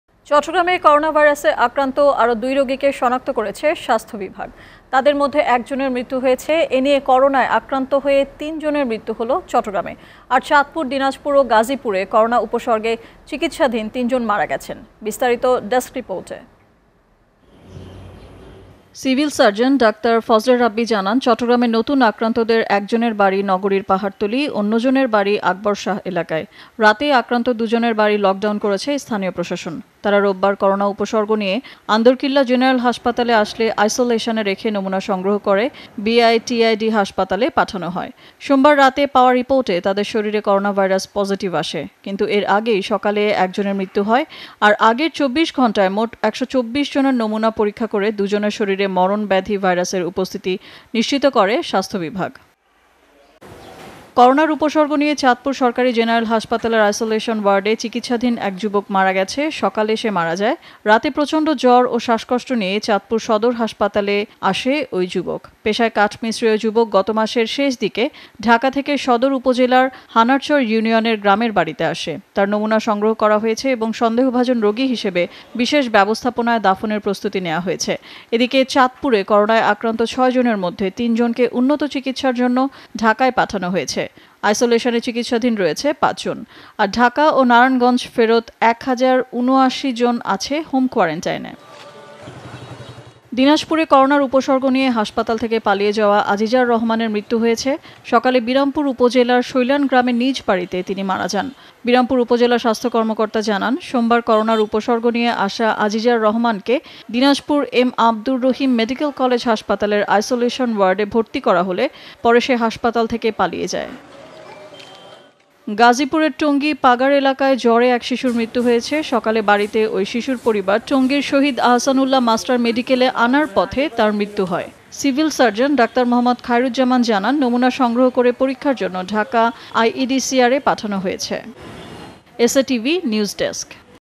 করোনায় আক্রান্ত হয়ে তিন জনের মৃত্যু হলো চট্টগ্রামে
SA TV Location:
Bangladesh